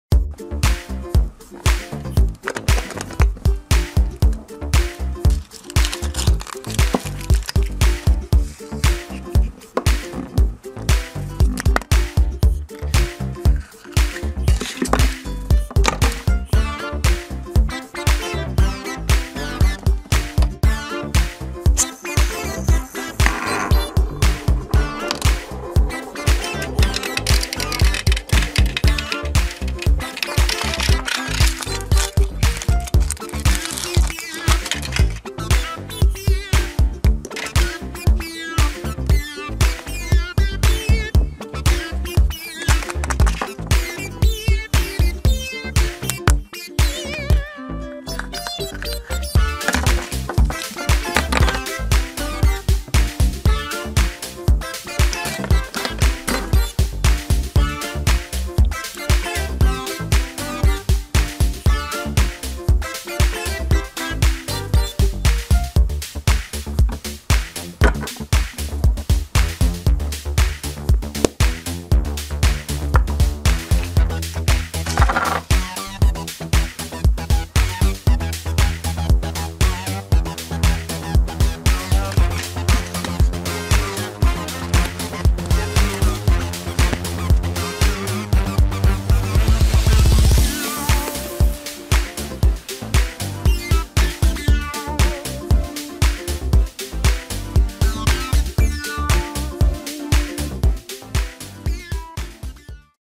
Unwrapping 3 Girl kinder Surprise Eggs Disney Princess
Unwrapping 3 Girl Surprise Eggs Disney Princess and two other surprises.